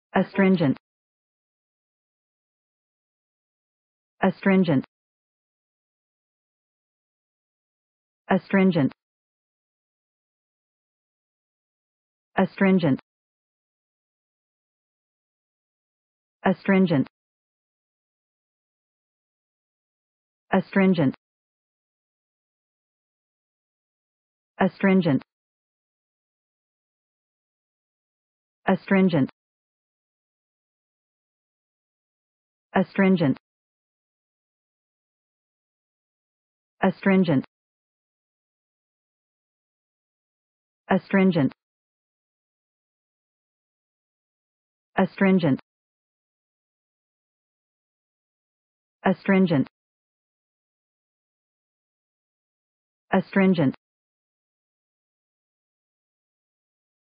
Pronunciation of Astringent | Definition of Astringent
Astringent pronunciation | How to pronounce Astringent in English?
/ə`strɪndʒənt/
Meaning of Astringent | What is Astringent?
(1) (adjective) sour or bitter in taste; (2) (noun) a drug that causes contraction of body tissues and canals; (3) (adjective) tending to draw together or constrict soft organic tissue;
What is Astringent | Astringent meaning and definition?